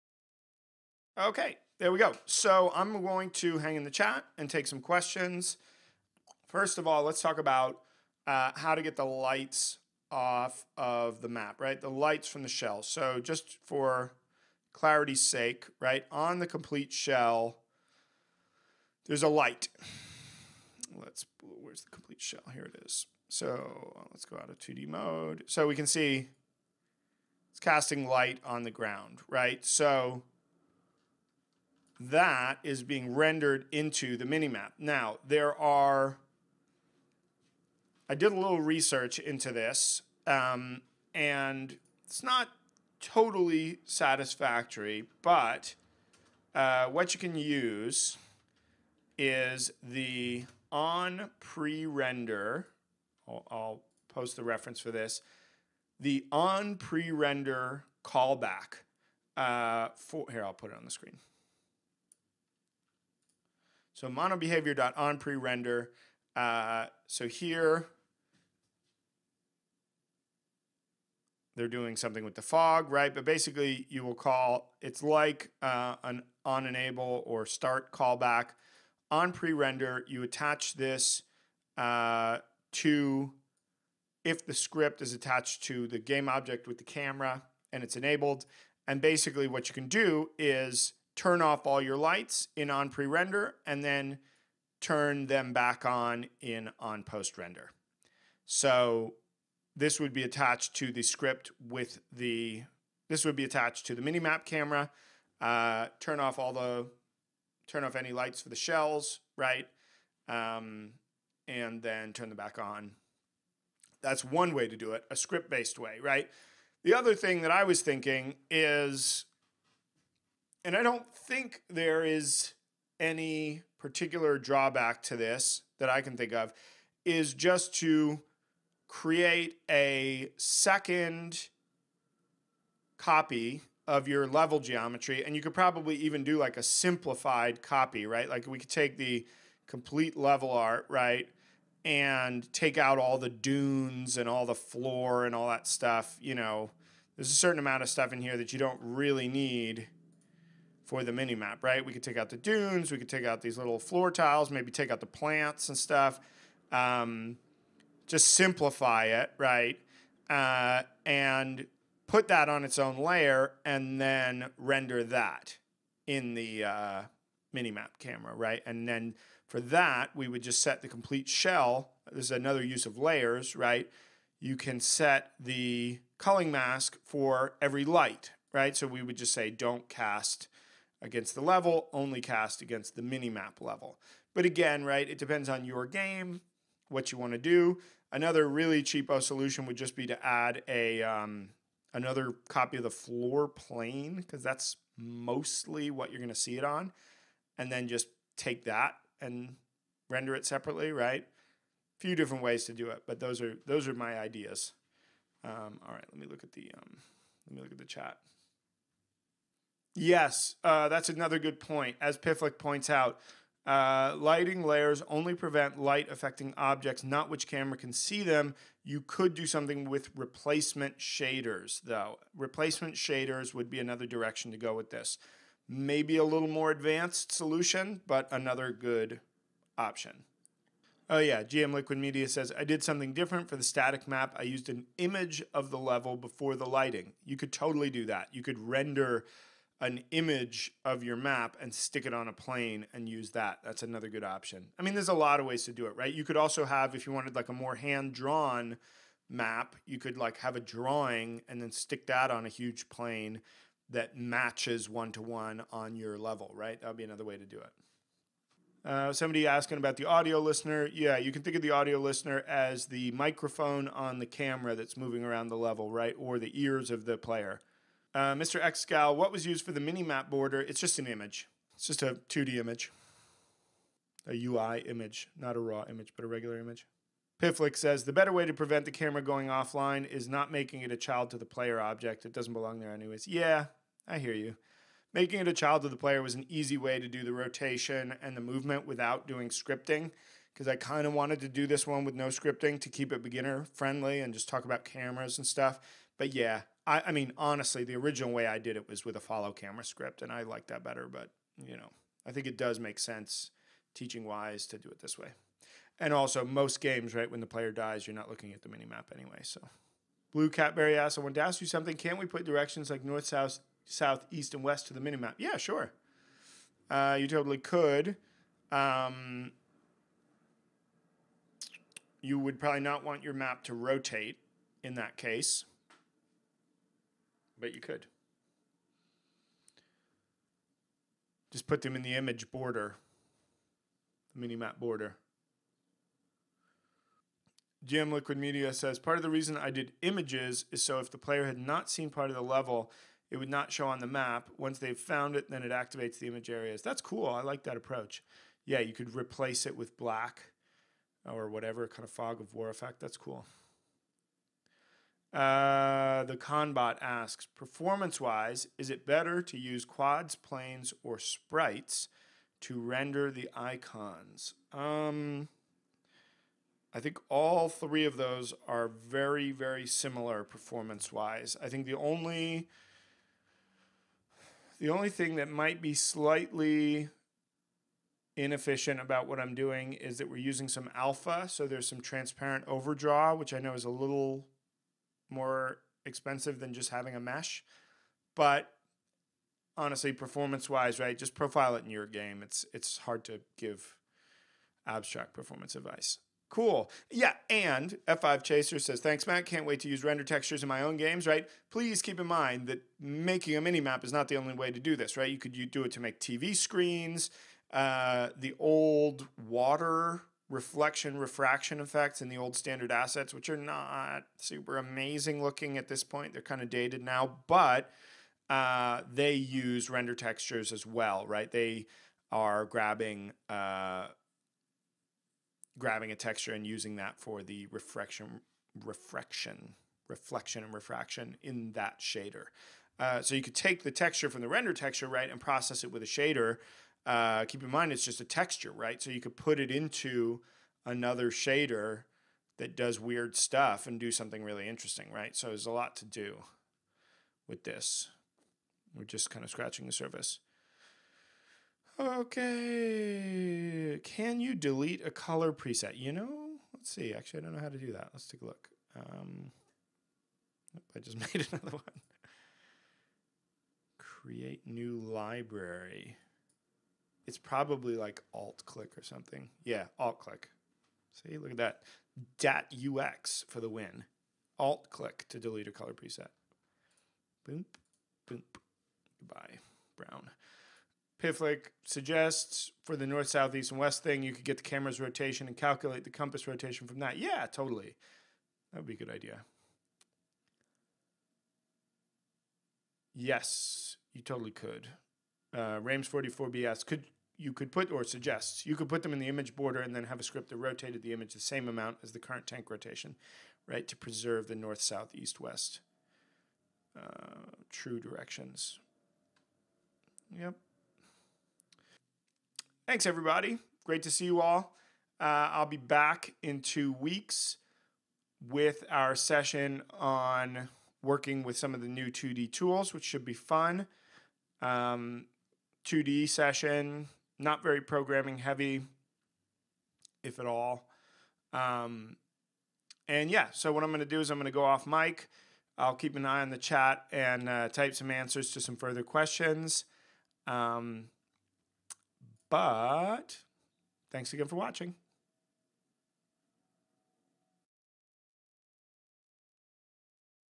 Creating A Minimap System - Questions and Answers [6/6] Live 2017/5/24
In this live training session we will learn how to create a minimap system which displays objects near the player as icons in a top down view. In this episode we will answer questions from guests in the live chat on Twitch.